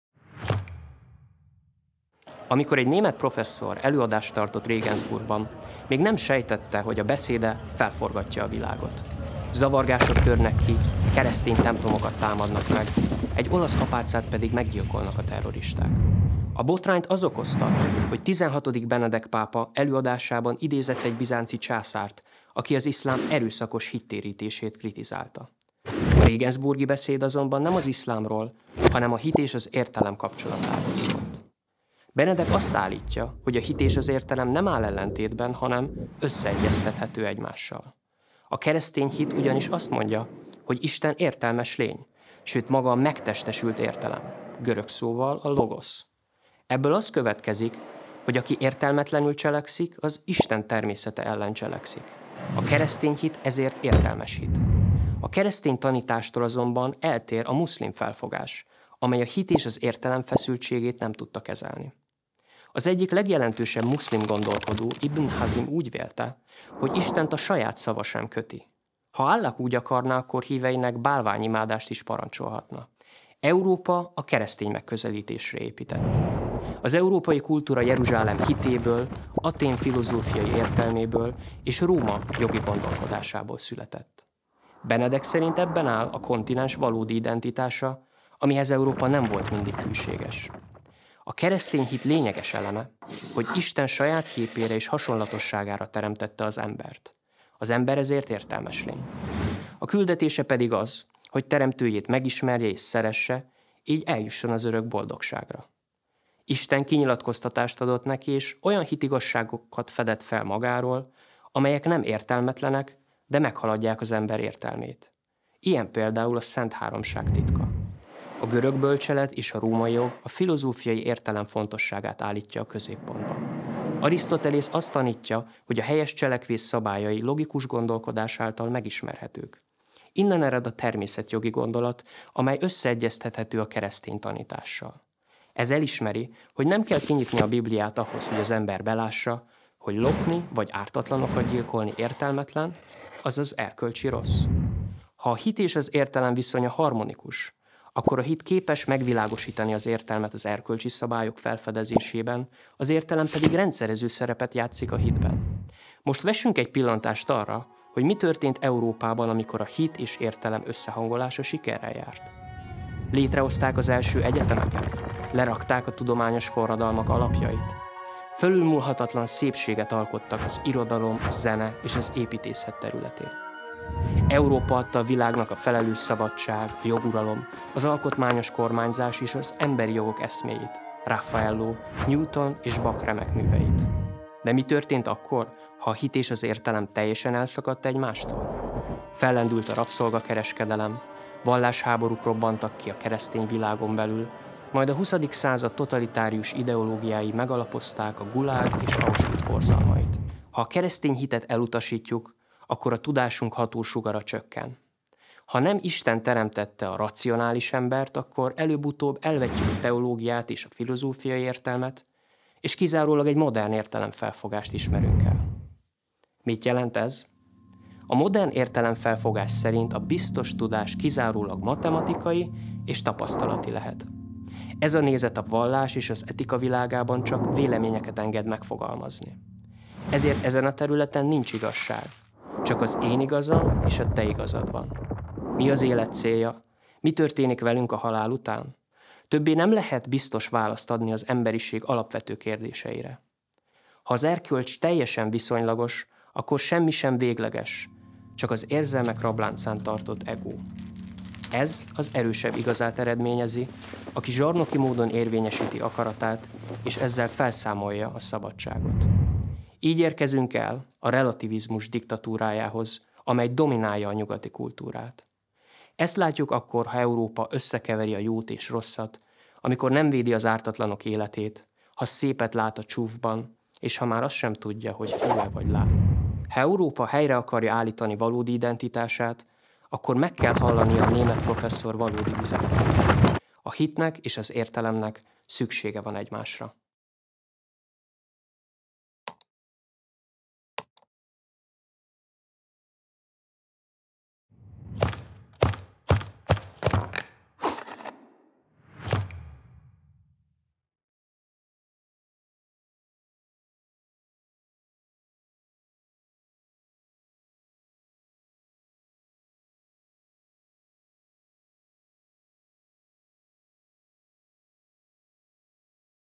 Milyen alapokra épült Európa?
A beszédről, ami megrengette a világot. Filemon Norbert az Axiómán. Nem hosszabb, mint egy espresso. ☕

Szöveg:

Amikor egy német professzor előadást tartott Regensburgban, még nem sejtette, hogy a beszéde felforgatja a világot. Zavargások törnek ki, keresztény templomokat támadnak meg. Egy olasz apácát pedig meggyilkolnak a terroristák.

A botrányt az okozta, hogy XVI. Benedek pápa előadásában idézett, egy bizánci császárt, aki az iszlám erőszakos hittérítését kritizálta. A regensburgi beszéd azonban, nem az iszlámról, hanem a hit és az értelem kapcsolatáról szólt. Benedek azt állítja, hogy a hit és az értelem nem áll ellentétben, hanem összeegyeztethető egymással.

A keresztény hit ugyanis azt mondja, hogy Isten értelmes lény, sőt maga a megtestesült értelem, görög szóval, a Logosz. Ebből az következik, hogy aki értelmetlenül cselekszik, az Isten természete ellen cselekszik. A keresztény hit, ezért értelmes hit.

A keresztény tanítástól azonban eltér a muszlim felfogás, amely a hit és az értelem feszültségét nem tudta kezelni. Az egyik legjelentősebb muszlim gondolkodó, Ibn Hazm, úgy vélte, hogy Istent a saját szava sem köti. Ha Allah úgy akarná, akkor híveinek bálványimádást is parancsolhatna.

Európa a keresztény megközelítésre épített. Az európai kultúra Jeruzsálem hitéből, Athén filozófiai értelméből és Róma jogi gondolkodásából született. Benedek szerint ebben áll a kontinens valódi identitása, amihez Európa nem volt mindig hűséges.

A keresztény hit lényeges eleme, hogy Isten saját képére és hasonlatosságára teremtette az embert. Az ember ezért értelmes lény. A küldetése pedig, az hogy Teremtőjét megismerje és szeresse, így eljusson az örök boldogságra. Isten kinyilatkoztatást adott neki, és olyan hitigazságokat fedett fel magáról, amelyek nem értelmetlenek, de meghaladják az ember értelmét. Ilyen például a Szentháromság titka.

A görög bölcselet és a római jog a filozófiai értelem fontosságát állítja a középpontba. Arisztotelész azt tanítja, hogy a helyes cselekvés szabályai logikus gondolkodás által megismerhetők. Innen ered a természetjogi gondolat, amely összeegyeztethető a keresztény tanítással. Ez elismeri, hogy nem kell kinyitni a Bibliát ahhoz, hogy az ember belássa, hogy lopni vagy ártatlanokat gyilkolni értelmetlen, azaz erkölcsi rossz.

Ha a hit és az értelem viszonya harmonikus, akkor a hit képes megvilágosítani az értelmet az erkölcsi szabályok felfedezésében. Az értelem pedig rendszerező szerepet játszik a hitben.

Most vessünk egy pillantást arra, hogy mi történt Európában amikor a hit és értelem összehangolása sikerrel járt?

Létrehozták az első egyetemeket. Lerakták a tudományos forradalmak alapjait. Felülmúlhatatlan szépséget alkottak az irodalom, a zene és az építészet területén. Európa adta a világnak a felelős szabadság, a joguralom, az alkotmányos kormányzás és az emberi jogok eszméit, Raffaelo, Newton és Bach remekműveit.

De mi történt akkor, ha a hit és az értelem teljesen elszakadt egymástól? Fellendült a rabszolga-kereskedelem, vallásháborúk robbantak ki a keresztény világon belül, majd a XX. század totalitárius ideológiái megalapozták a Gulág és Auschwitz borzalmait.

Ha a keresztény hitet elutasítjuk, akkor a tudásunk hatósugara csökken. Ha nem Isten teremtette a racionális embert, akkor előbb-utóbb elvetjük a teológiát és a filozófiai értelmet, és kizárólag egy modern értelemfelfogást ismerünk el.

Mit jelent ez?

A modern értelemfelfogás szerint a biztos tudás kizárólag matematikai és tapasztalati lehet. Ez a nézet, a vallás és az etika világában csak véleményeket enged megfogalmazni. Ezért ezen a területen nincs igazság. Csak az én igazam és a te igazad van.

Mi az élet célja? Mi történik velünk a halál után? Többé nem lehet biztos választ adni az emberiség alapvető kérdéseire.

Ha az erkölcs teljesen viszonylagos, akkor semmi sem végleges, csak az érzelmek rabláncán tartott egó. Ez az erősebb igazát eredményezi. Aki zsarnoki módon érvényesíti akaratát, és ezzel felszámolja a szabadságot.

Így érkezünk el a relativizmus diktatúrájához, amely dominálja a nyugati kultúrát. Ezt látjuk akkor ha Európa összekeveri a jót és rosszat. Amikor nem védi az ártatlanok életét. Ha szépet lát a csúfban, és ha már azt sem tudja, hogy fiú-e vagy lány.

Ha Európa helyre akarja állítania valódi identitását, akkor meg kell hallania a német professzor valódi üzenetét. A hitnek és az értelemnek szüksége van egymásra.